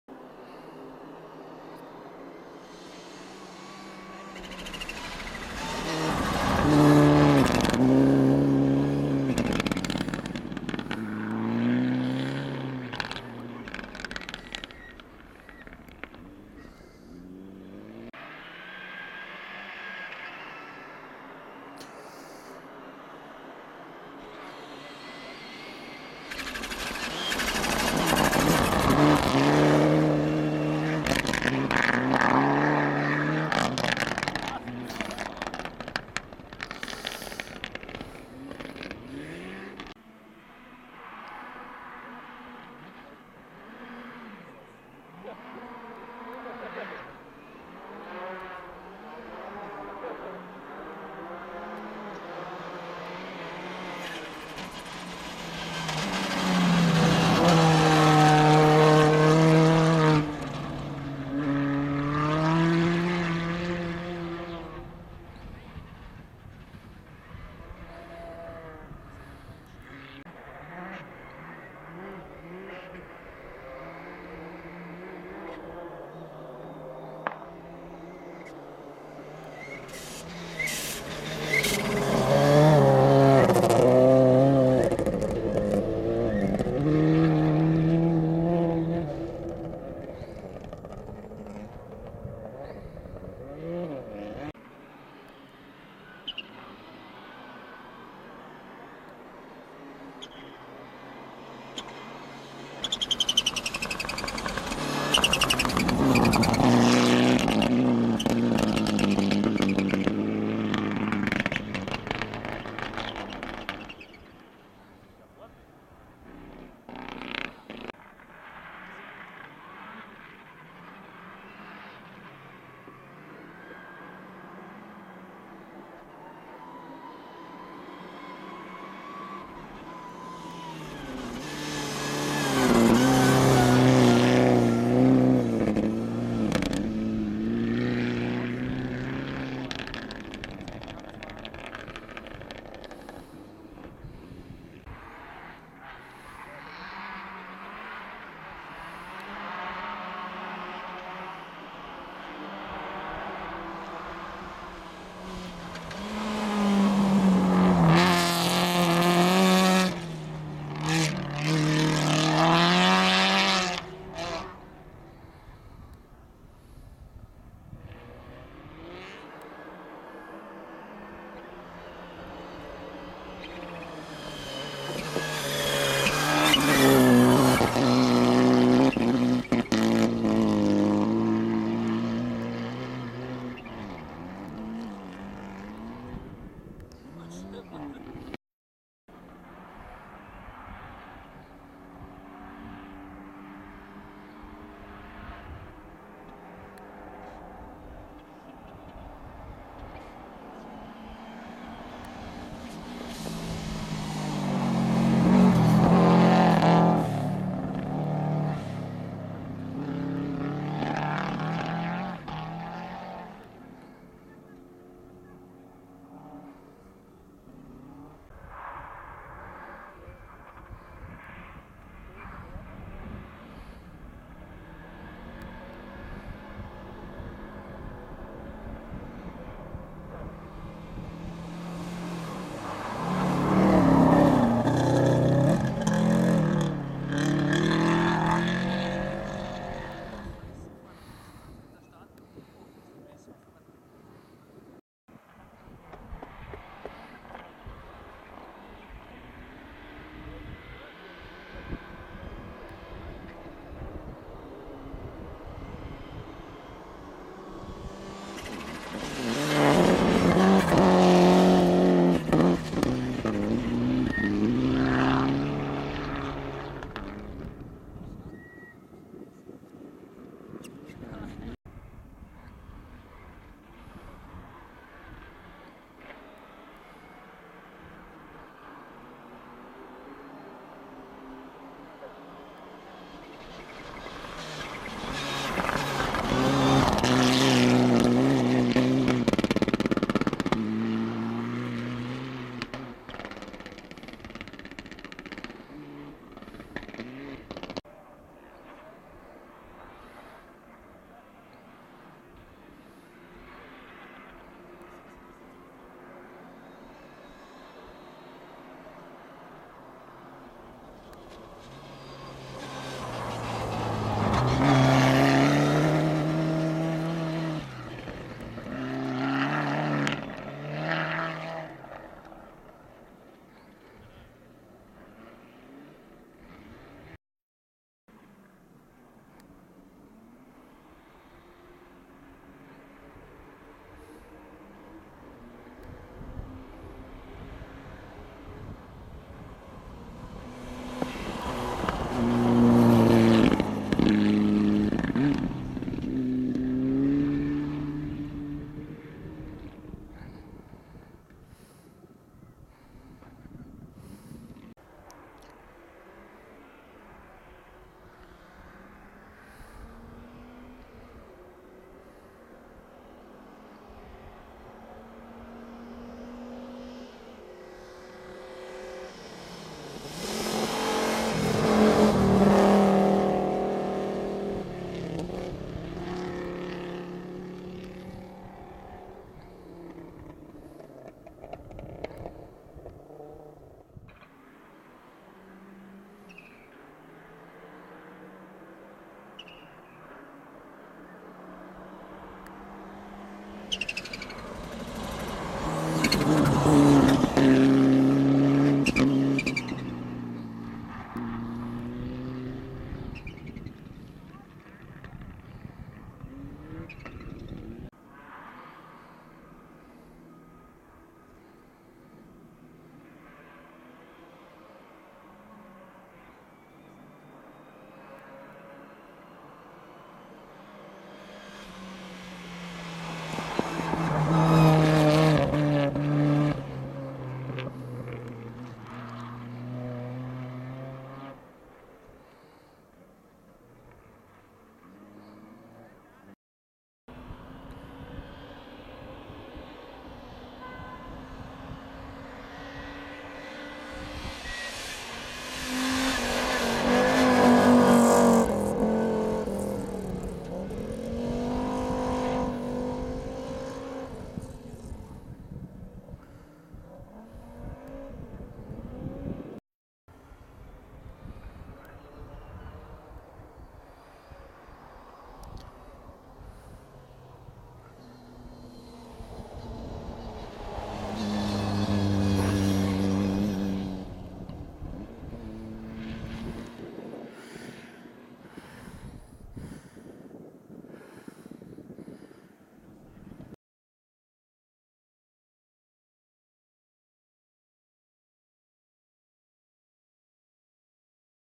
Jänner Rallye 2019 SP 3 St. Oswald Nr. 11-30 Teil 2
Sonderprüfung 3 der Jännerrallye 2019 wurde in Obermarreith,  St. Oswald gestartet. Bergaufstart und ein lange Gerade.